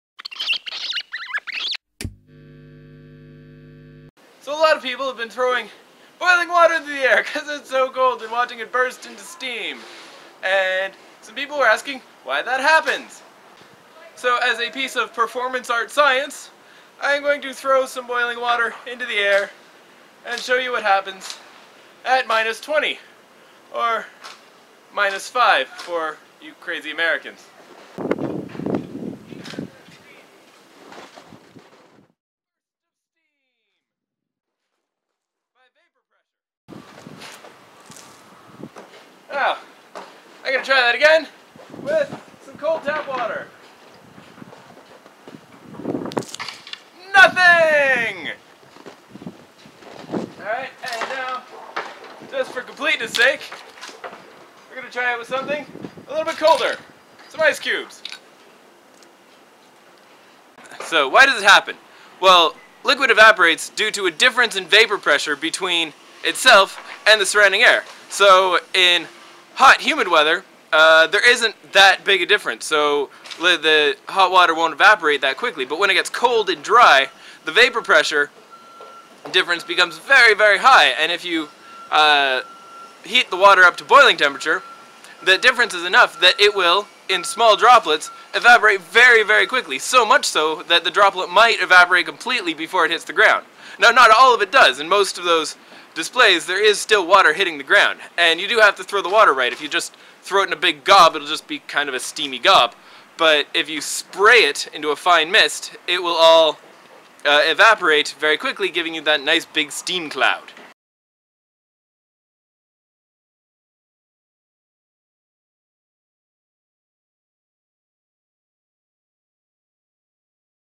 Cold Steam
Today I went outside in the horrible cold to throw water in the air for science. The results are misty and kind of neat.

Apologies for the abbreviated explanation. It was cold outside.